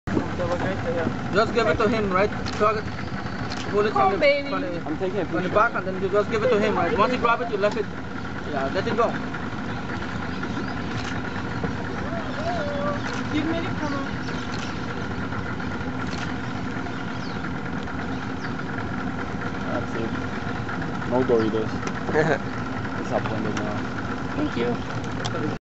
Spider Monkey Eating a Banana on New River
On the way to Lamanai Mayan Archaeological Site we ran into this monkey who loves his bananas.